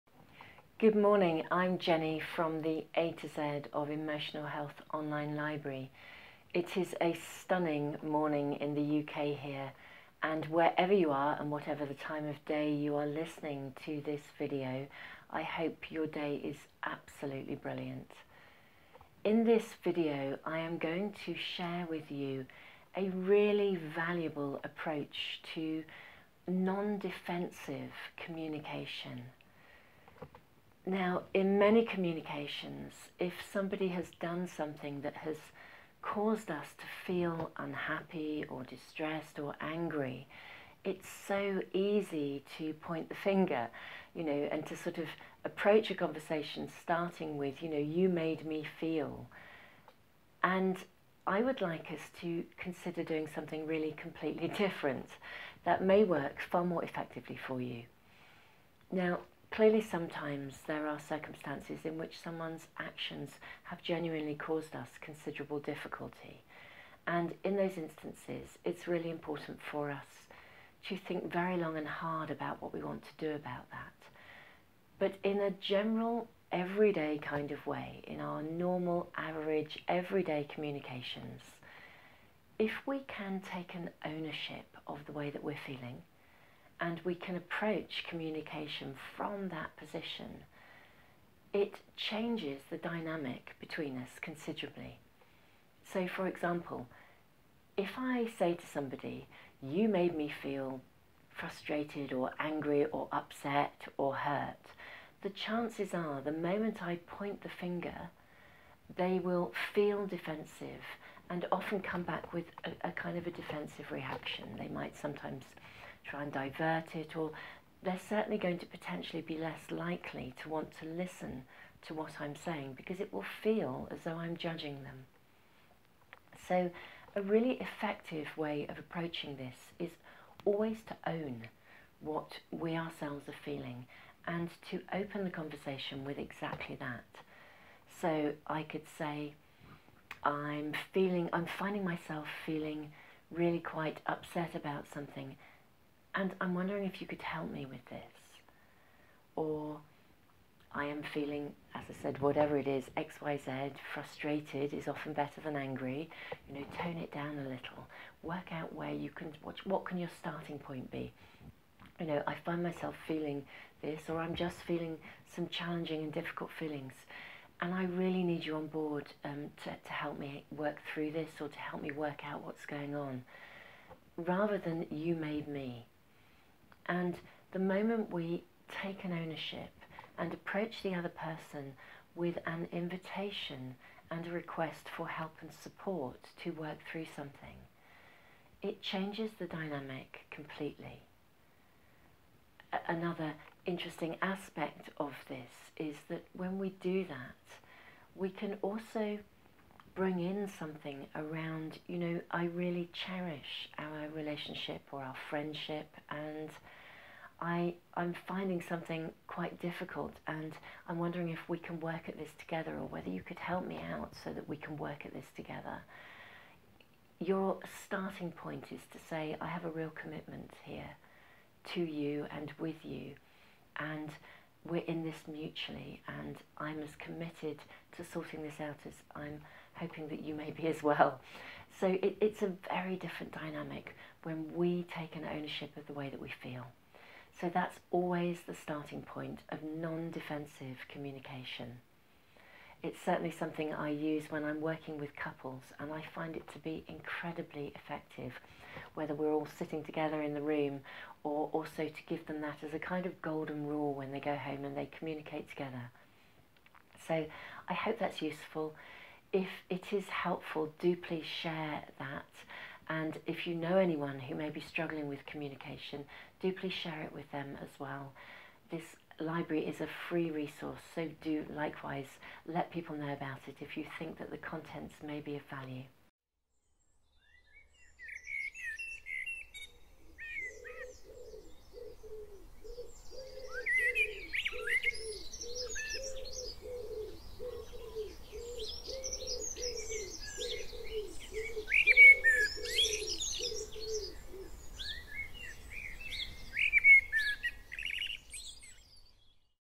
Non Defensive Communication - a Brilliant and Helpful Tool
An effective tool to help with ownership of our own feelings and to communicate these with others. This video is stored in the A-Z of Emotional Health on-line Library, a FREE public resource